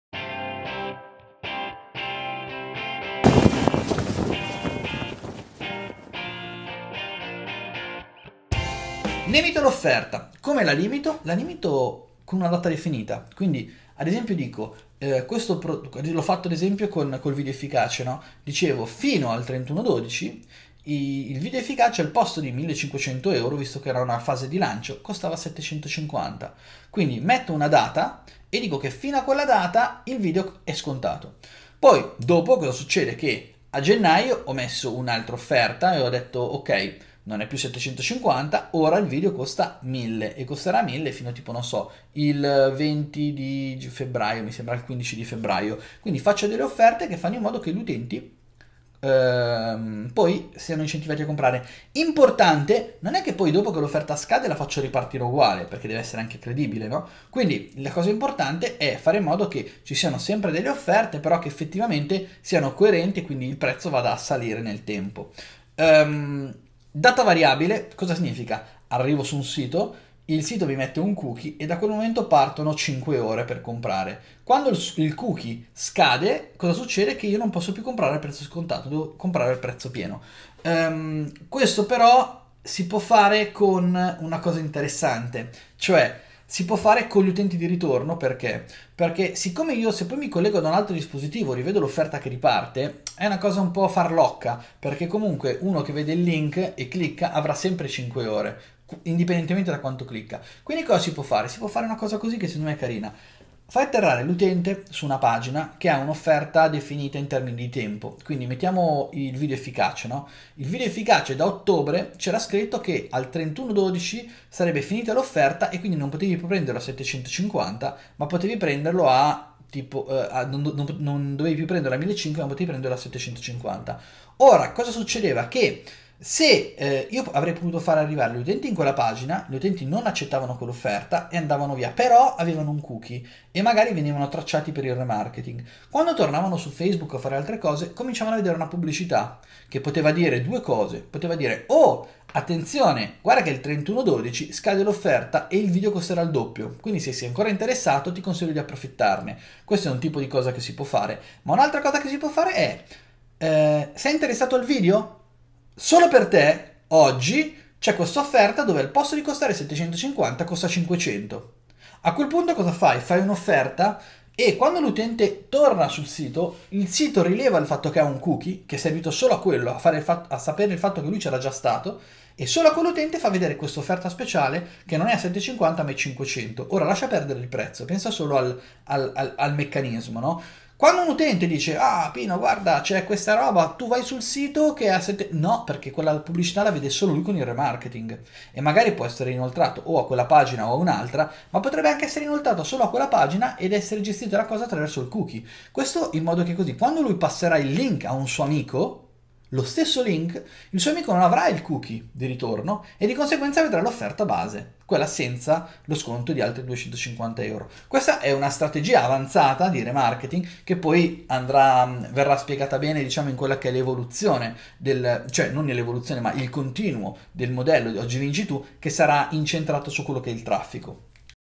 12 - Come convertire in prima visita?
Creando urgenza!
Sono certo che ti sarai imbattuto in uno o più marketers che utilizzano questa tecnica, magari un po' goffamente con una offerta scontata del 90% per poche ore...

Usando la creatività è possibile farlo in modo più efficace e credibile, ecco alcune idee!

Cosa aspetti?